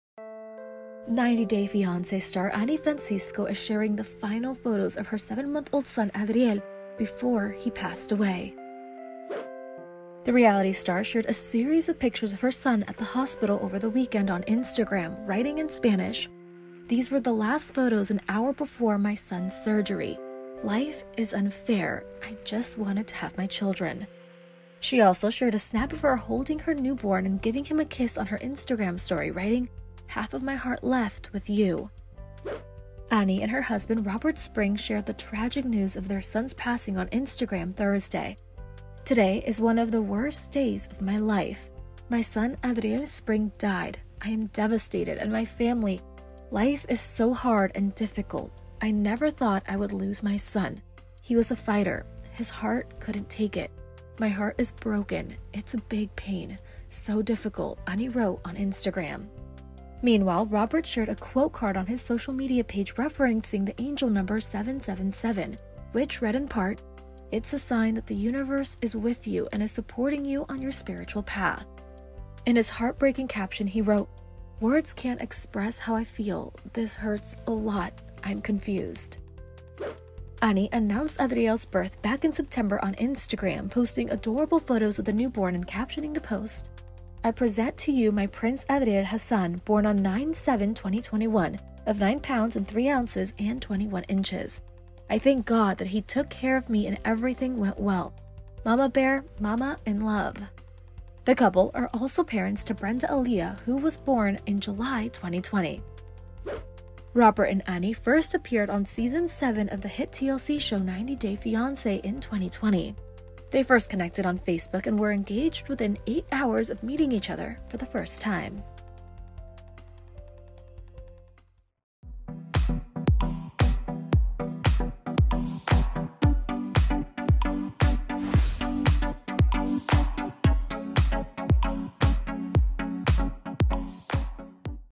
'90 Day Fiancé' Star Anny Francisco Shares Final Photos Of Baby Prior To His Death
"90 Day Fiancé" star Anny Francisco is sharing the final photos of her 7-month-old son, Adriel, before he passed away. The reality star shared a series of pictures of her son at the hospital over the weekend on Instagram writing in Spanish, "These were the last photos an hour before my son's surgery. Life is unfair I just wanted to have my children." Anny and her husband, Robert Springs shared the tragic news of their son's passing on Instagram Thursday.